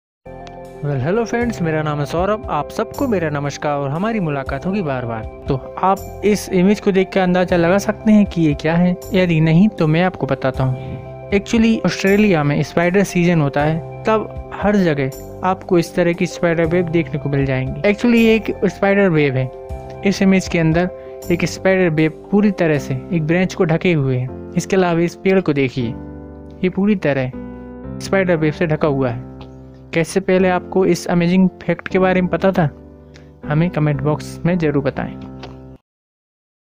amazing fact about austrelia | spider web | hindi
mr 100
amaging fact
dosto es video me mene apko autrelia me home wale spider season ke bare me bataya hai